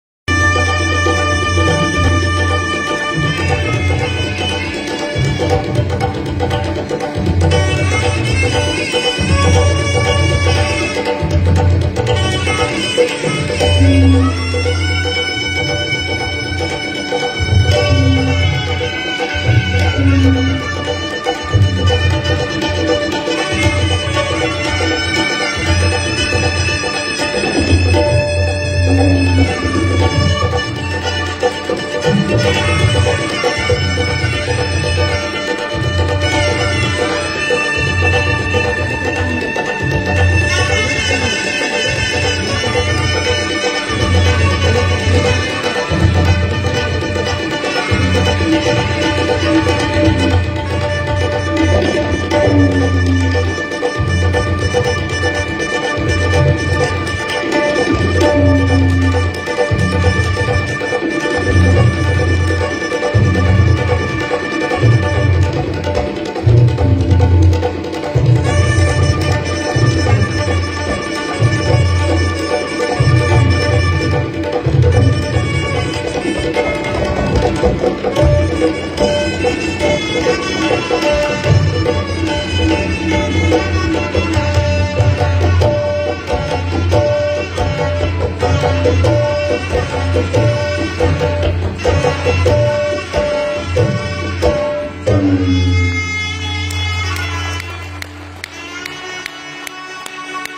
Raag Yaman |PADMASHRI Pandit Dr.S.Ballesh Bhajantri & Dr.Krishna Ballesh |Gaanayogi Swar Utsav 2020
Shehnai recital by
Padmashri Pandit Dr.S.Ballesh Bhajantri &
Kalaimamani Awardee Dr. Krishna Ballesh in Gaanayogi Swar Utsav 2022 in Kala Bhavan, Dharwad, India.

Accompanied by
Vidwan Suresh Raj - Tabla
Sri. Anagad Desai - Tabla
Dr. Prakash Ballesh - Dukkad
Dr. Shivanand Ballesh - Sur Shehnai
Sri. Sitar RK. Ravi Kumar - Swarmandal
Sri. Selva Murgan - Surpeti